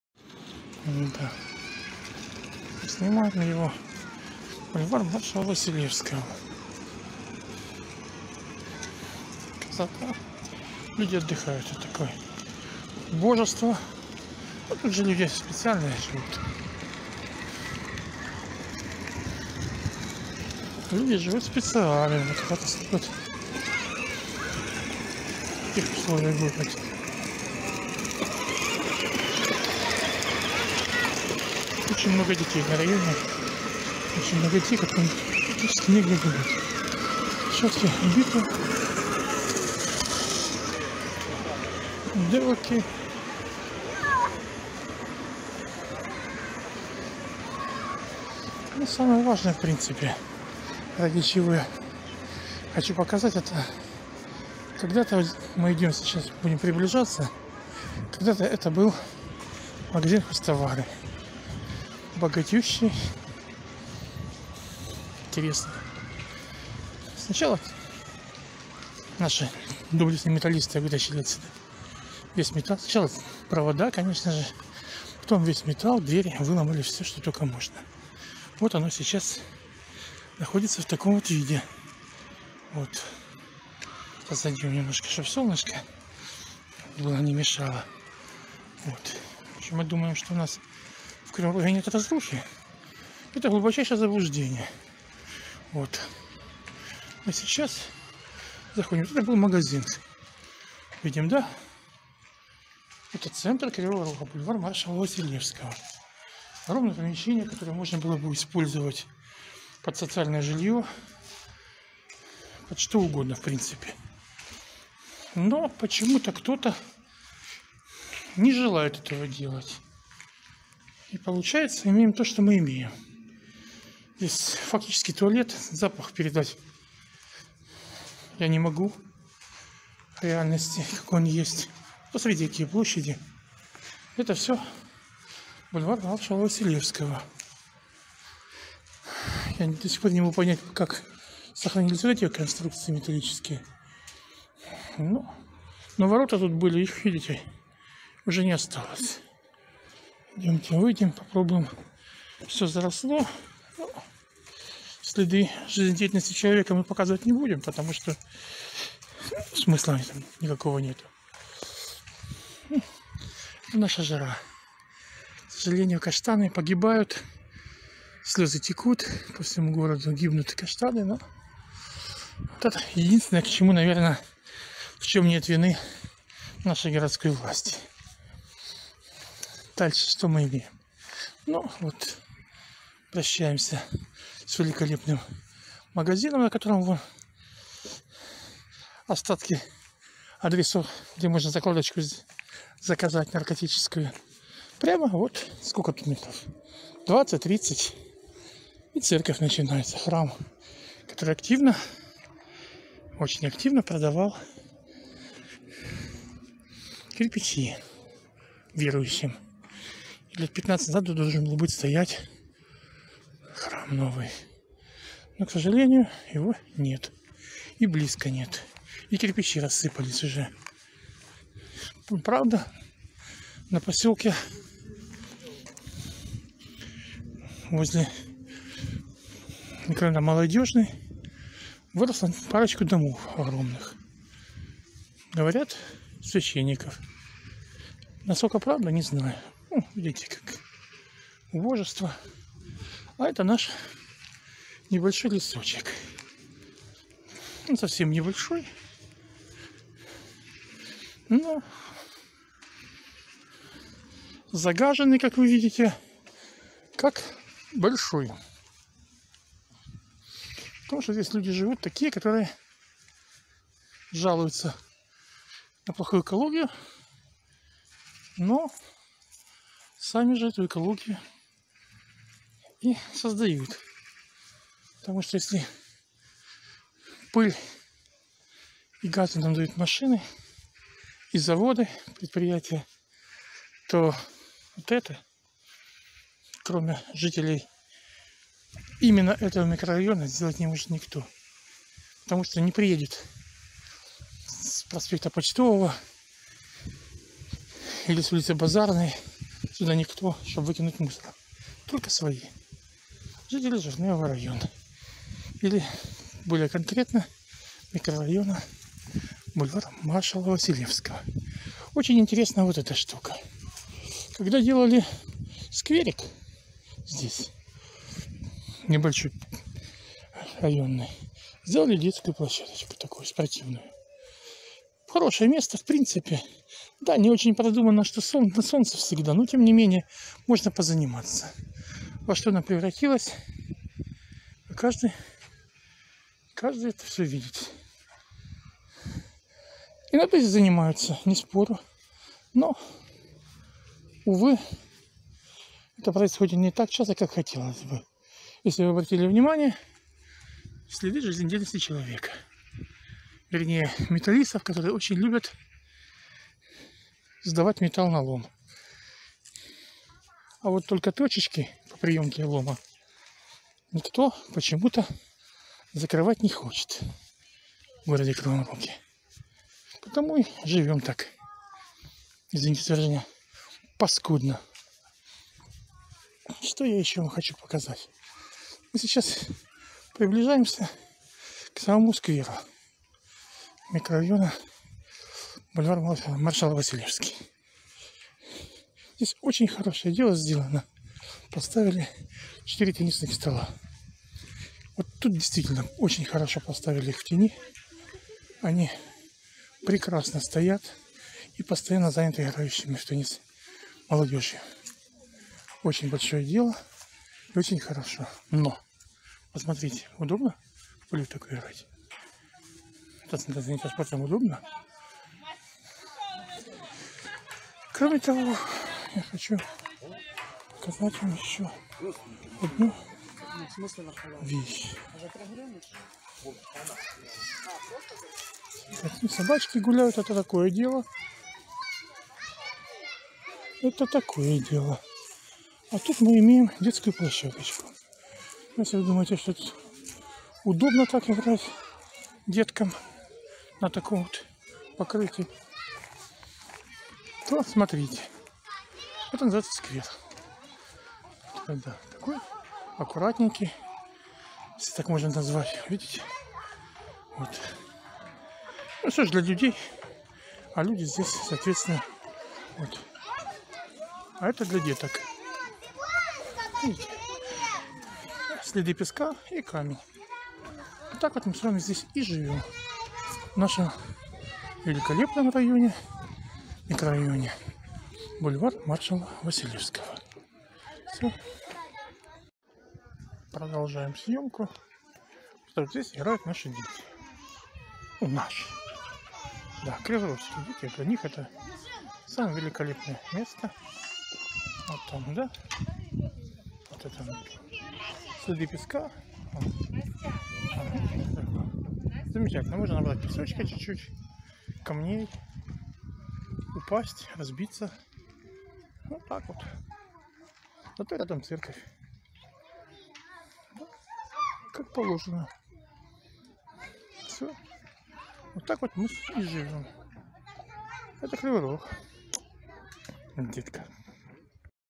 Убогий Кривой Рог Бульвар Маршала Василевского
Нам говорят, что Кривой Рог - город для людей. Вот только жители Бульвара Маршала Василевского (Покровский район) этого совсем не ощущают.
Представленное широкой публике видео снималось около 18-00 часов 9 сентября 2020 года.